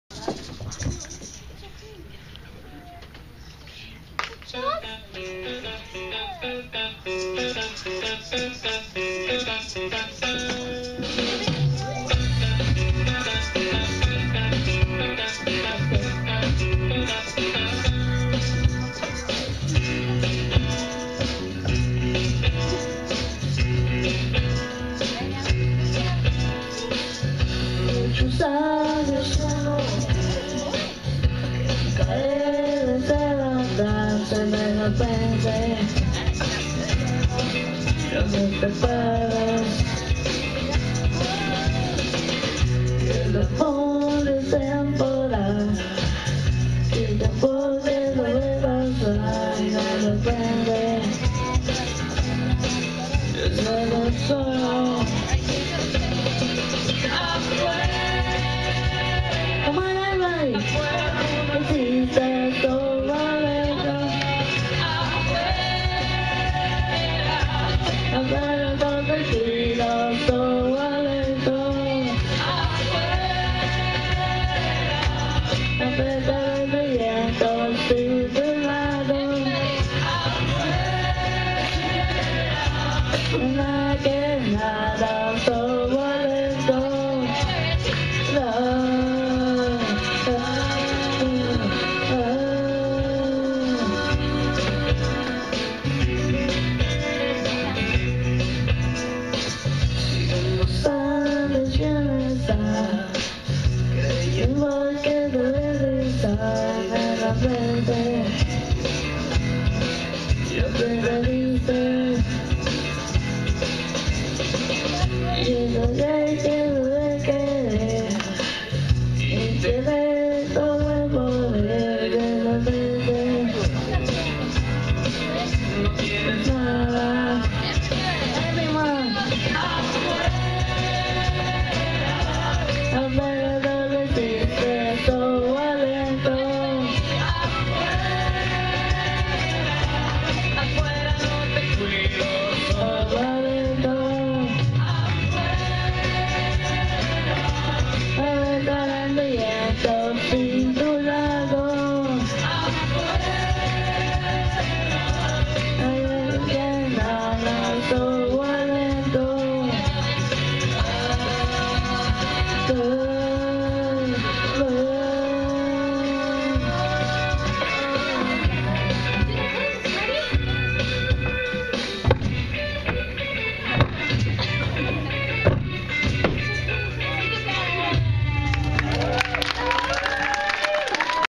Holiday Recital 2014: Alan
Alan sings an awesome rock n' roll tune on Friday night of the PCDA Holiday Recital!